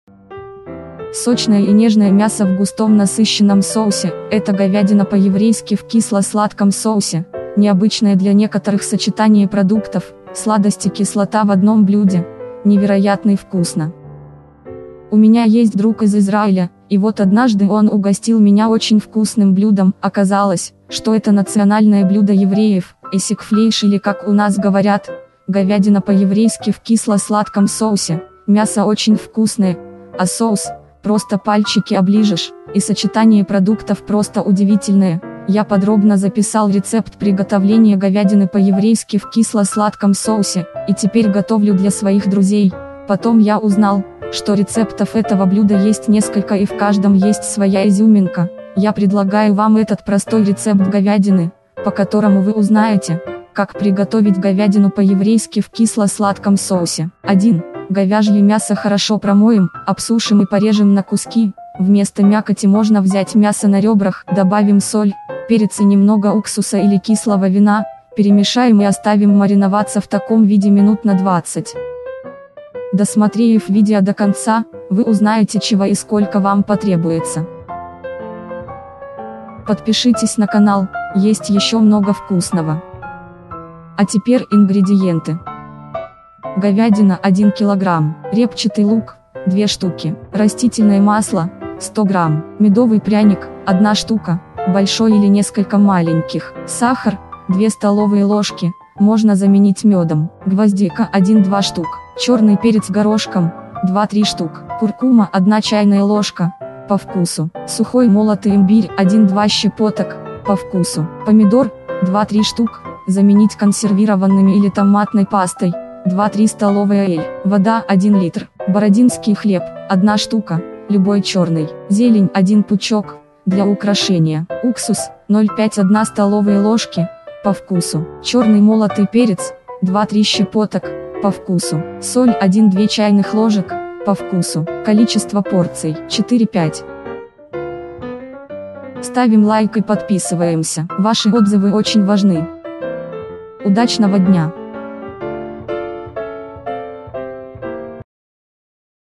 ★ ГОВЯДИНА ПО-ЕВРЕЙСКИ В КИСЛО-СЛАДКОМ СОУСЕ ★
★Вкус - божественный.★ Сочное и нежное мясо в густом насыщенном соусе - это говядина по-еврейски в кисло-сладком соусе.★ Необычное для некоторых сочетание продуктов, сладость и кислота в одном блюде - невероятный вкусно!